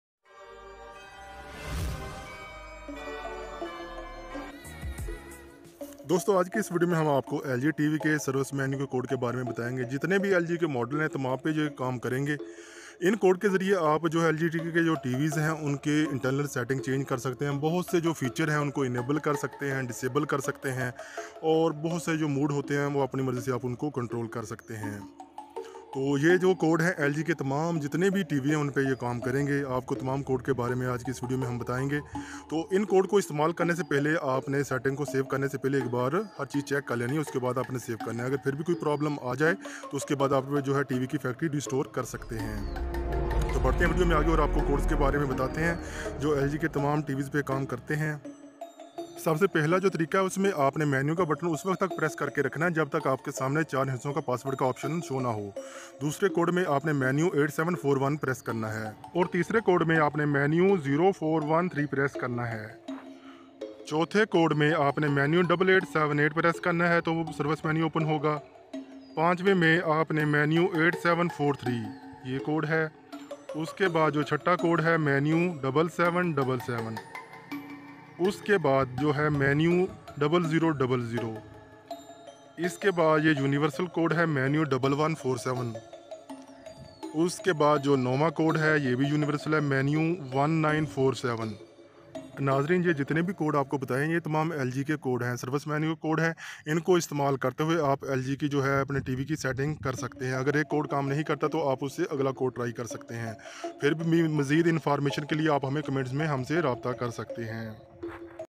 LG TVs Service Menu Access| How To Open Service Menu On All LG TV and LCD TV
How to access service menu on LG TVs and LG LCD TV and Led lg tv, Open LG TV Service Menu Codes Step by step instructions and guidance.
LG all models Tv service menu without remote control and with remote control.

Service menu on LG TV and LCD TV.